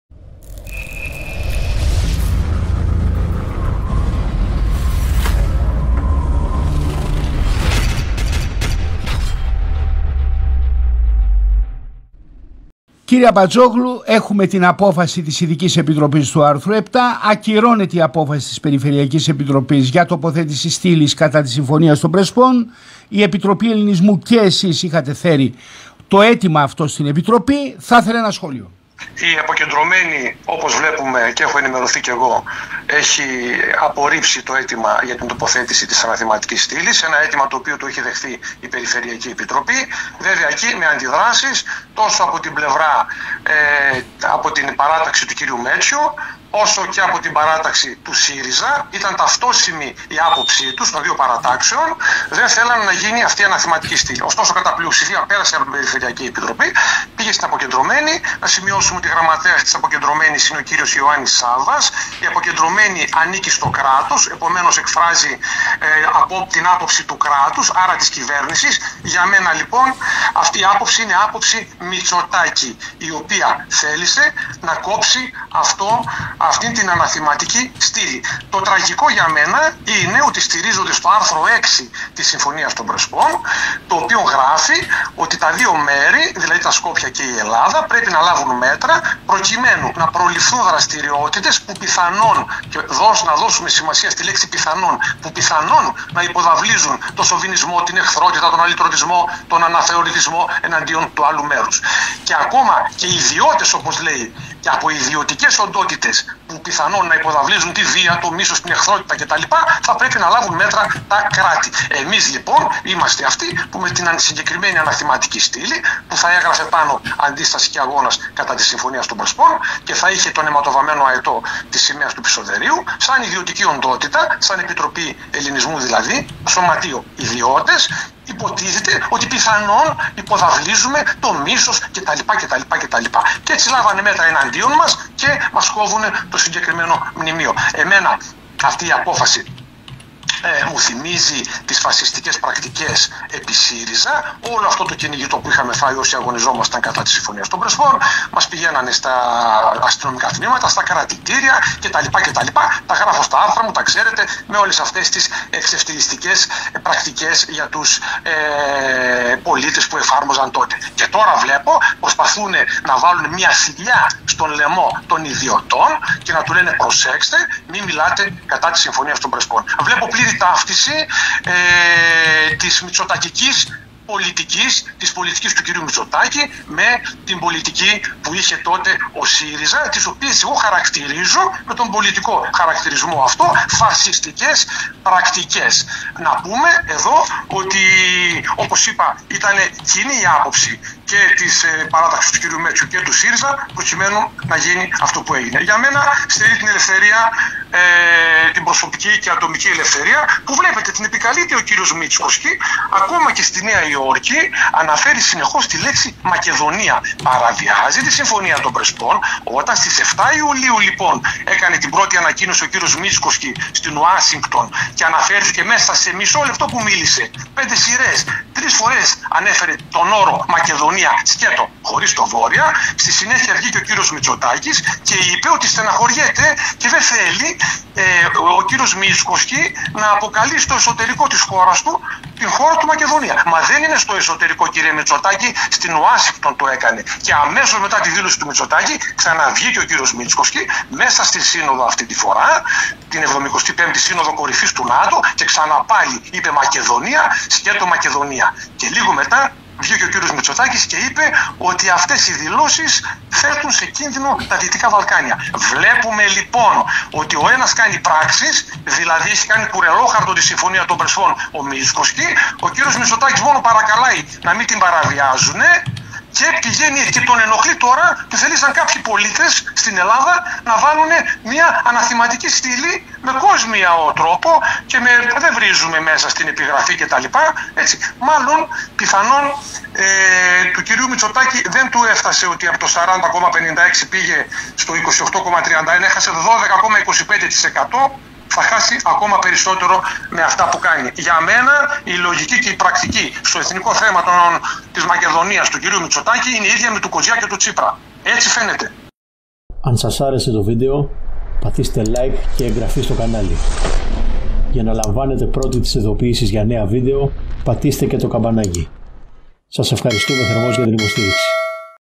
Απαγόρευσαν το μνημείο κατά της Συμφωνίας των Πρεσπών - Ιωάννης Αμπατζόγλου
Πηγή: Επιτροπή Ελληνισμού (Committee of Hellenism) - 16/07/24

➙ Ενισχύστε την προσπάθειά μας κάνοντας:
▶️ Like 👍 στο βίντεο.
☑️ Κοινοποιήσε το βίντεο
➙ Πατήστε το καμπανάκι 🔔 για να ενημερώνεστε για τα νέα μας βίντεο.